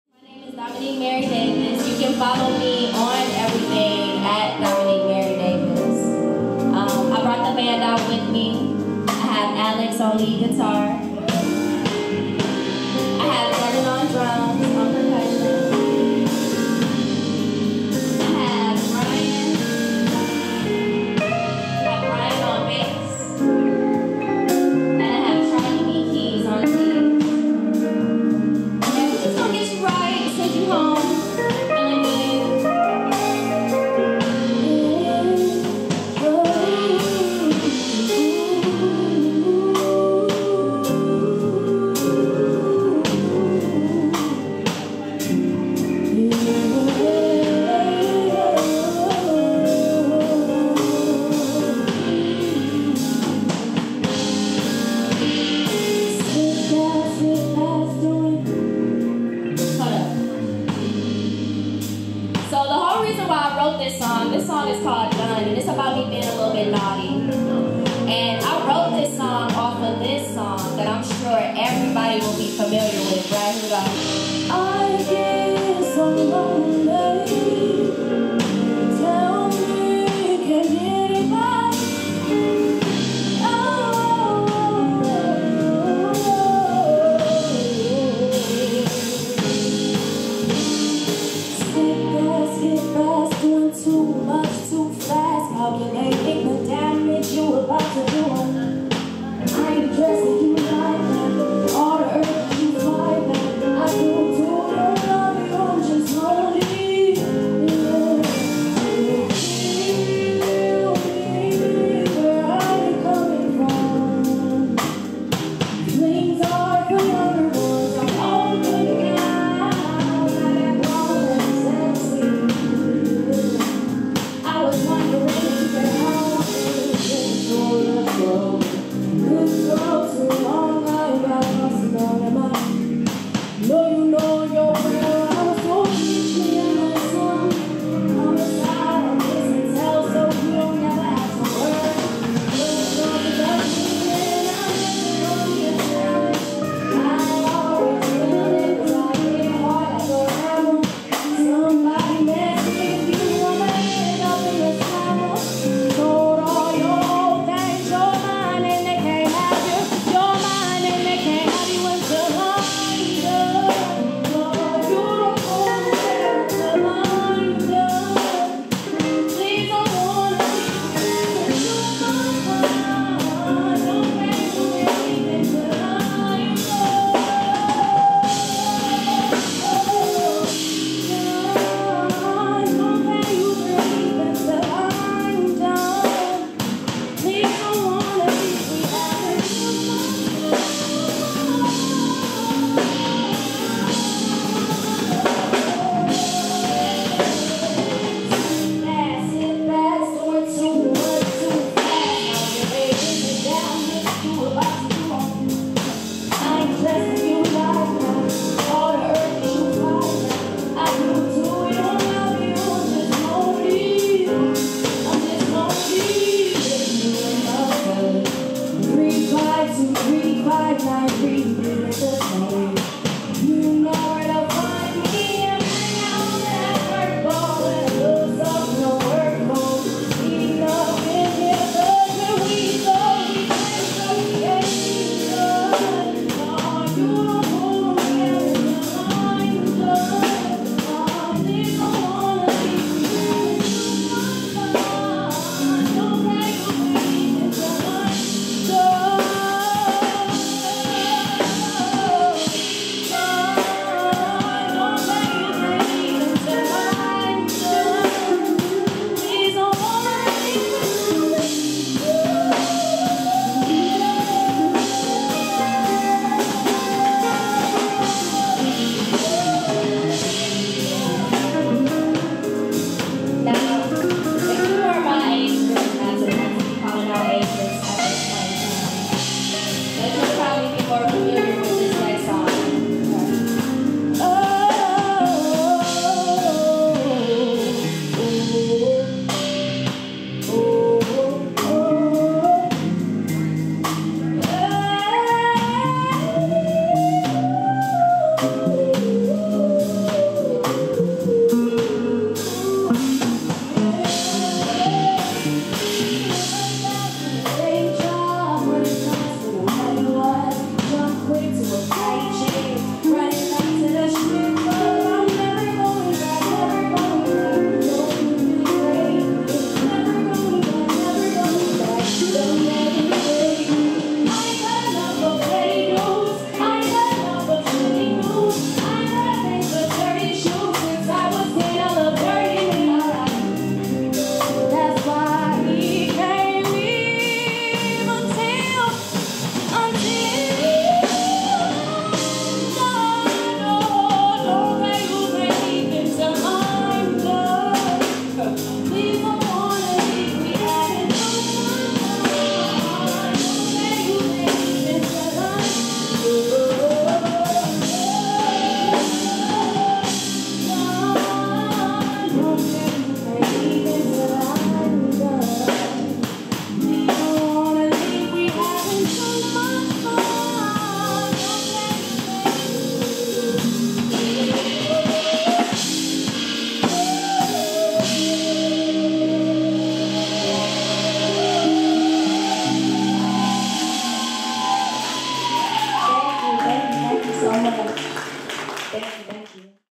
Dominique Mary Davis - Sounds of Growth DSC Performance 1
Live Performance by R&B Vocalist Dominique Mary Davis